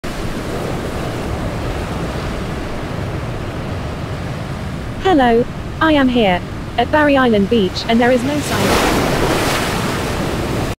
Girl Gets Knocked Out by Fish - Remake
A quick experiment to remake the famous video: "Girl gets knocked out by fish" by Kinging-it